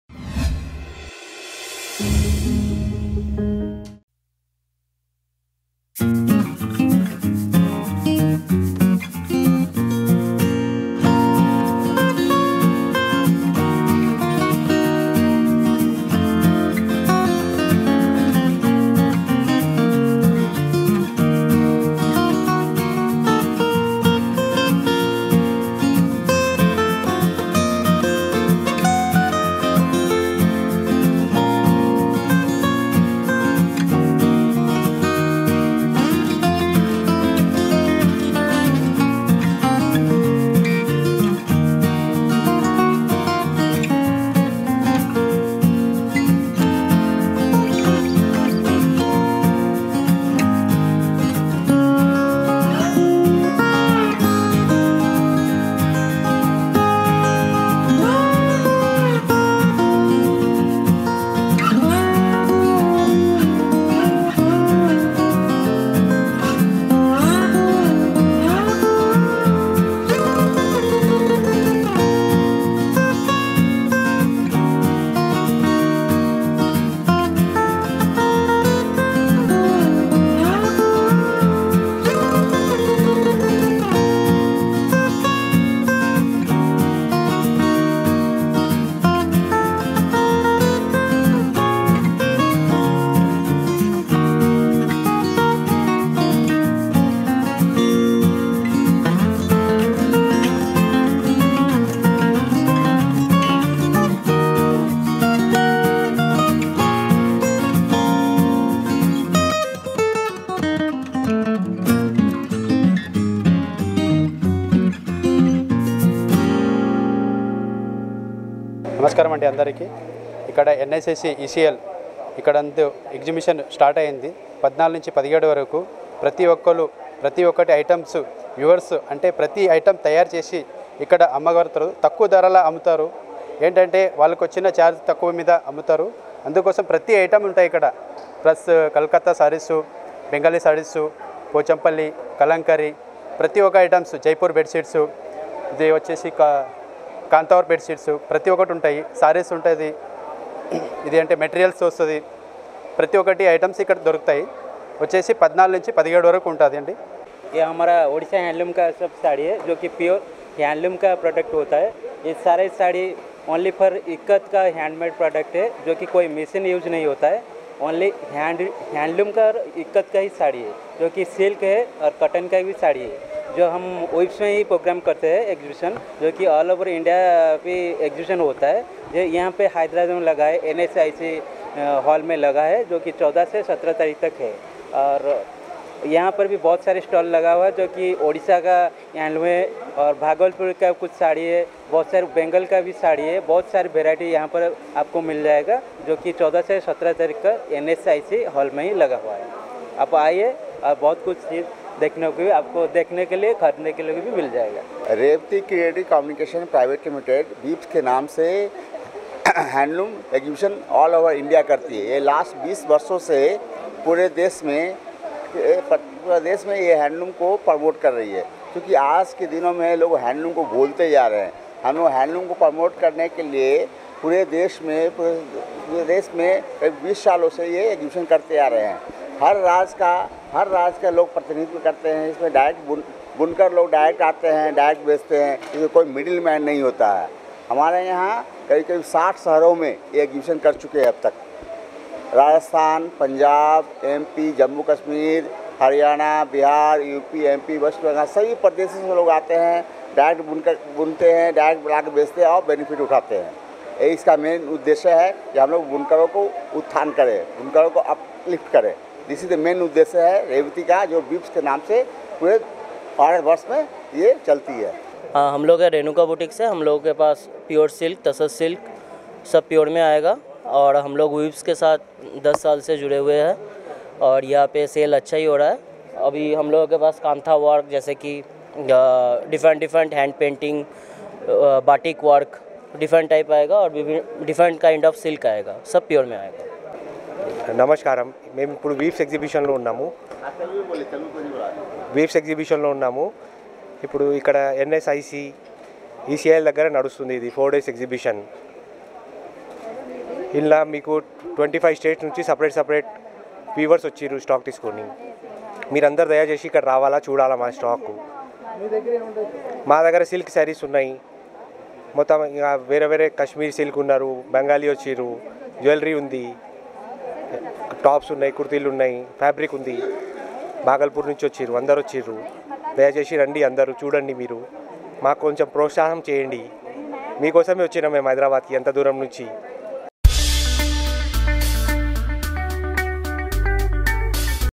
Weaves Exposition Of Handlooms at NSIC ECIL | Feb to 17 | zoneadds.com
The 'Weaves Exposition of Handlooms' is an initiative taken to promote hand woven cloth and clothing in India. The Expo, which took place from February 14th to 17th at the NSIC, ECIL, saw a lot of people coming in to have a look at the high quality, yet reasonably priced clothing and accessories. Zone Adds covers the expo with this short video. Have a look to see the quality materials that flew into town from various cities across our country!